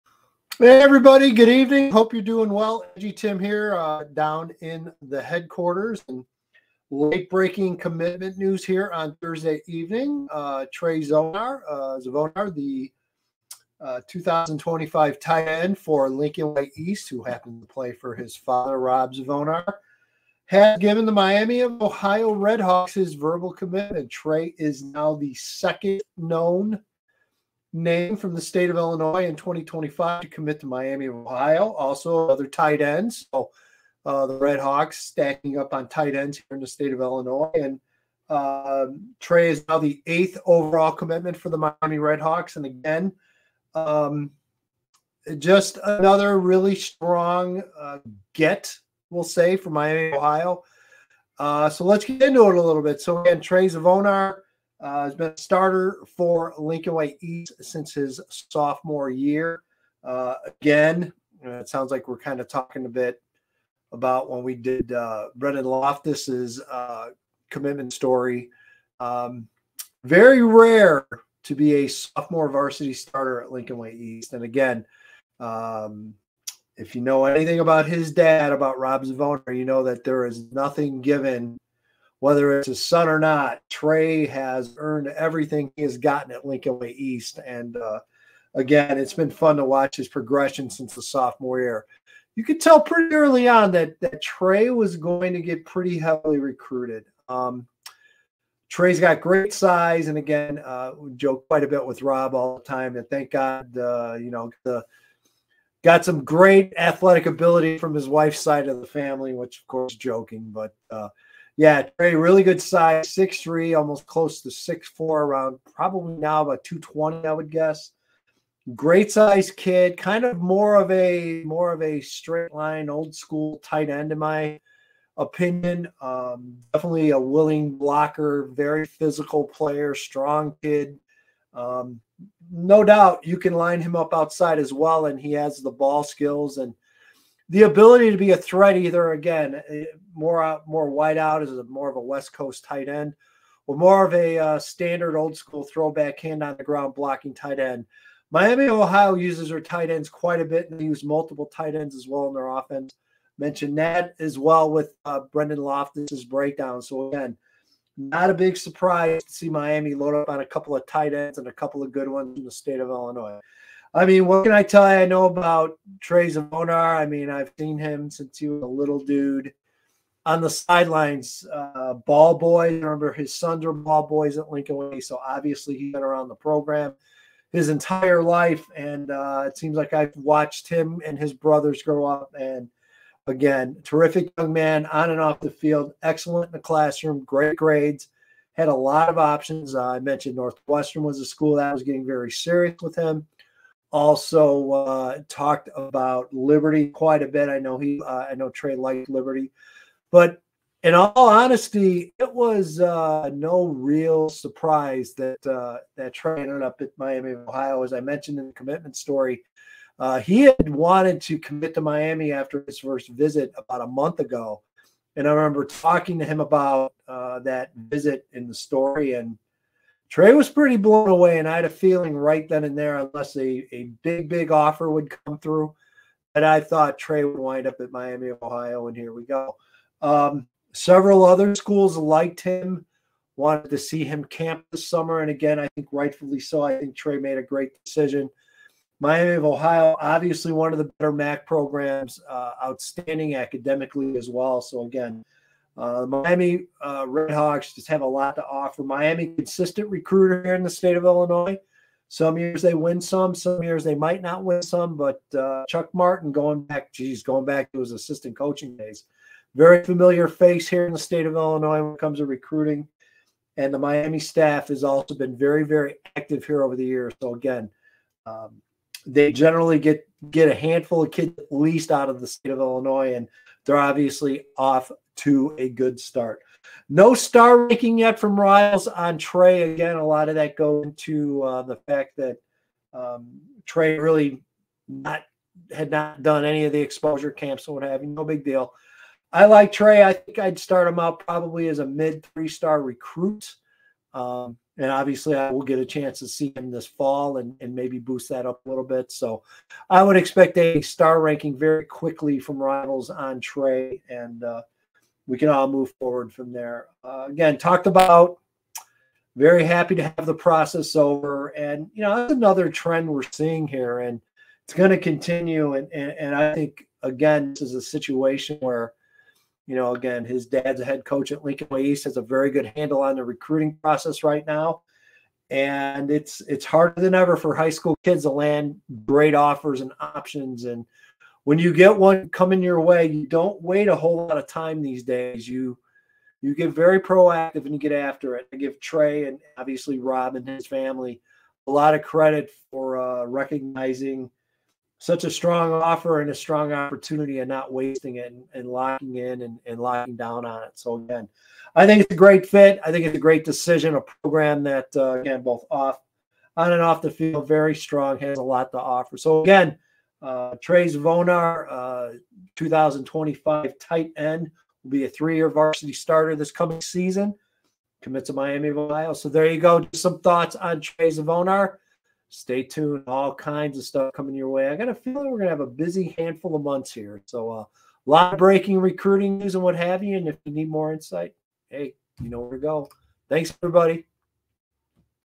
My Take LW East TE Trey Zvonar to Miami of Ohio
EDGYTIM discusses what the Miami of Ohio Redhawks are getting in Lincoln Way East 2025 TE Trey Zvonar